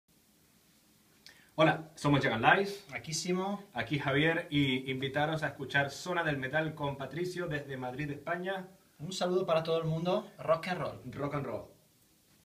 ZONA DEL METAL MADRID ESPAÑA-SALUDO DE JACK'N'LIES
Saludo efusivo  de los madrileños JACK'N'LIES para nuestro programa